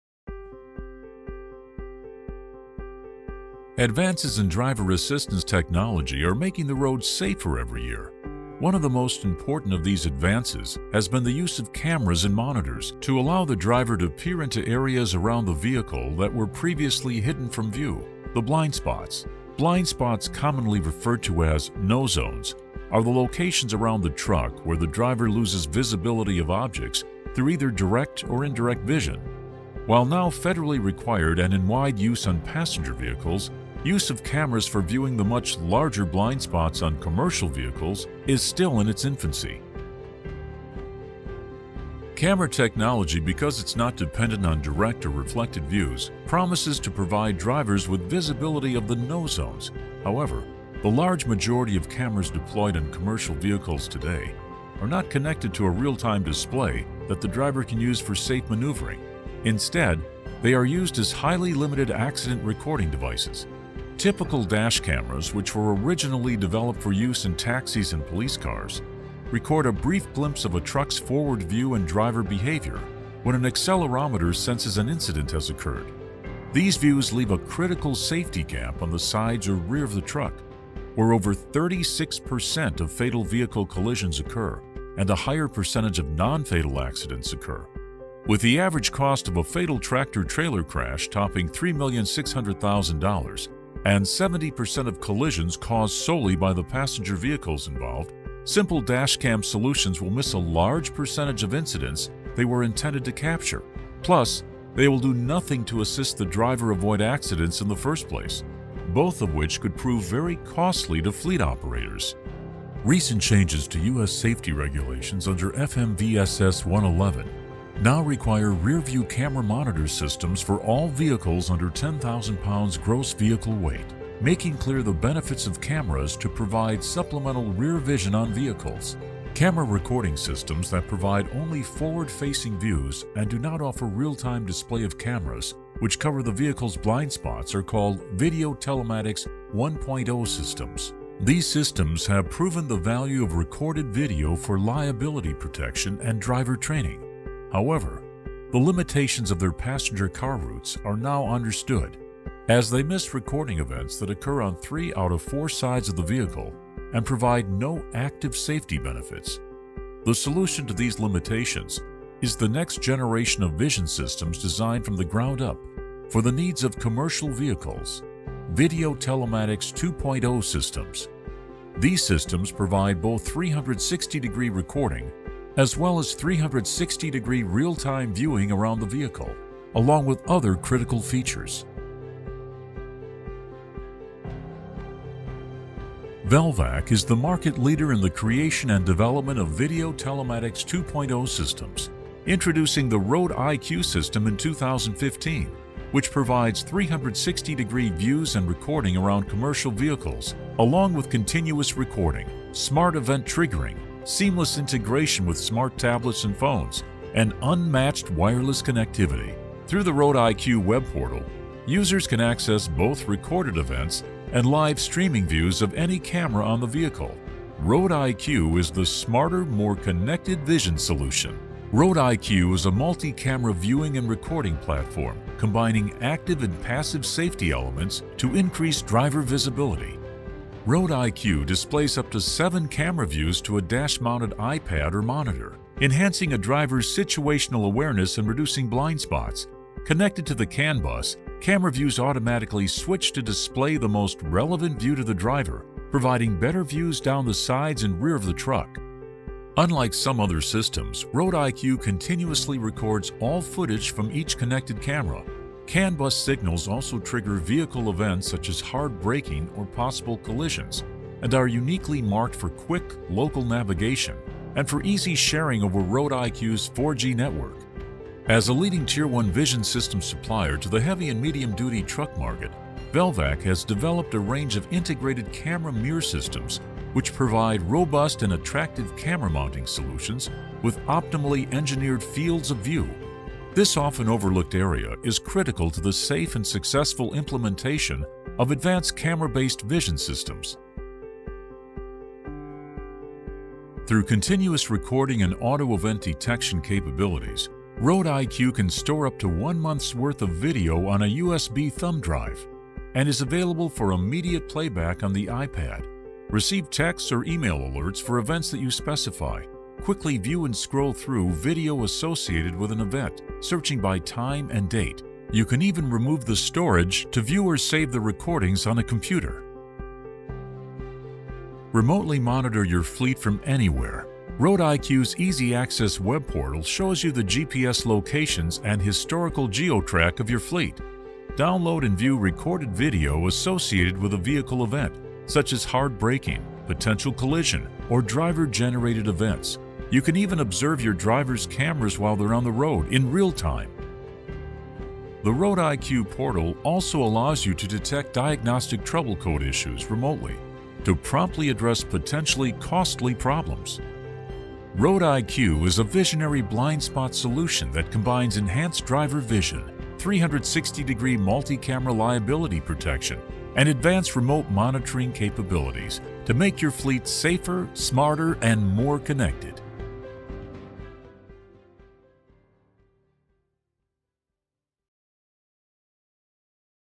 Road-iQ - Reinventing Video Telematics
Road-iQ is reinventing the way drivers see and record their blindspots, to simultaneously increase awareness and reduce liability.  Road-iQ's other unique features such as 4G connectivity, vehicle diagnostics, and remote live viewing, make it the ultimate solution for companies and individuals looking to increase connectivity with their vehicle(s).

Road-iQ provides both active and passive safety to your vehicle.